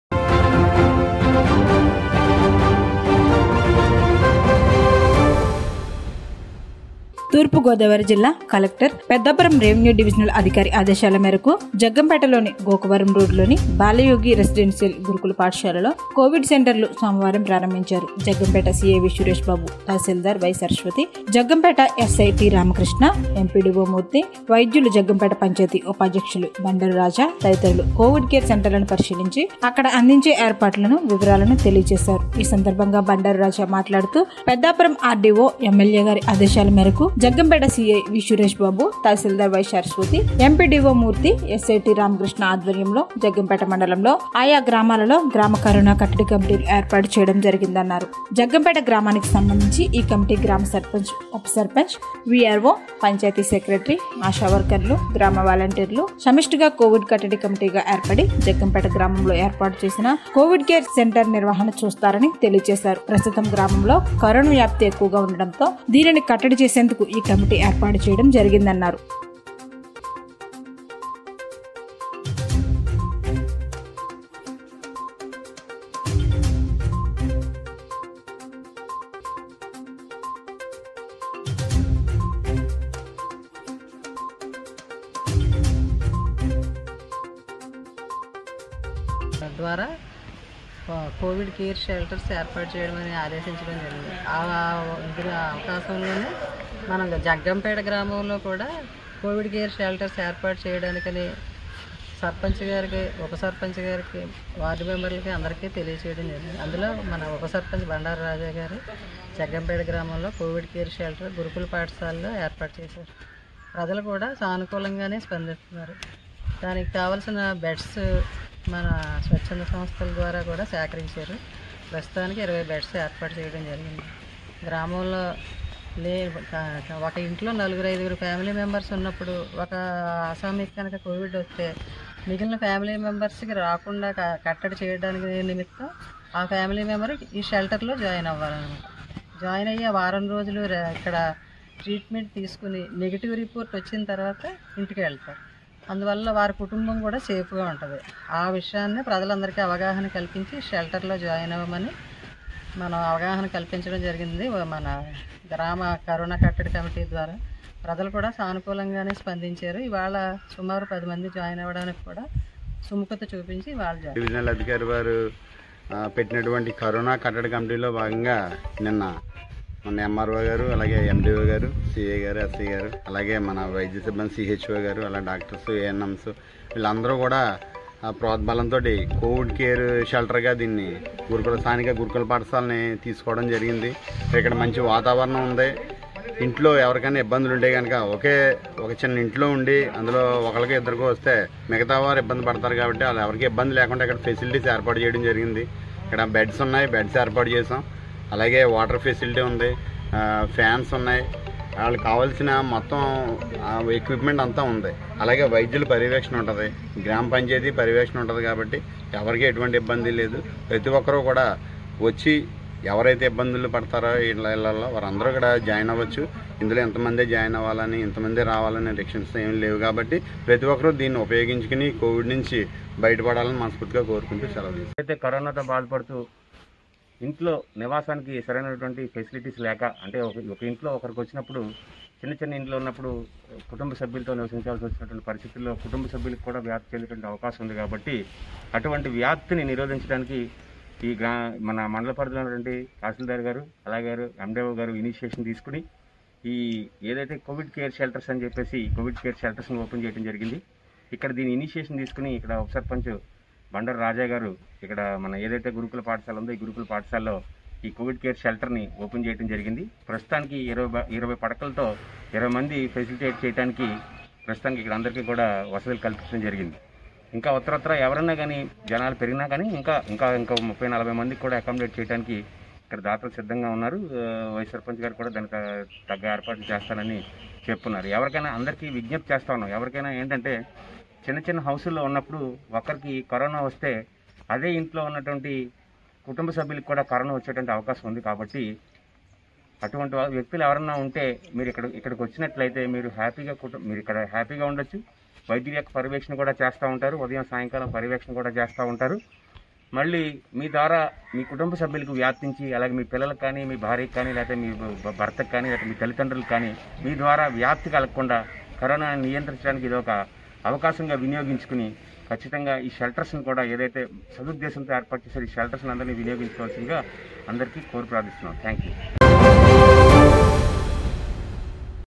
తూ.గో.జిల్లా:జగ్గంపేటలో గోకవరం రోడ్డులోని బాలయోగి రెసిడెన్షియల్ గురుకుల పాఠశాల కోవిడ్ సెంటర్ గా
తూర్పుగోదావరి జిల్లా:కలెక్టర్.పెద్దాపురం రెవెన్యూ డివిజనల్ అధికారి ఆదేశాల మేరకు జగ్గంపేట లోని గోకవరం రోడ్డులోని బాలయోగి రెసిడెన్షియల్ గురుకుల పాఠశాలలో కోవిడ్ సెంటర్ ను సోమవారం ప్రారంభించారు.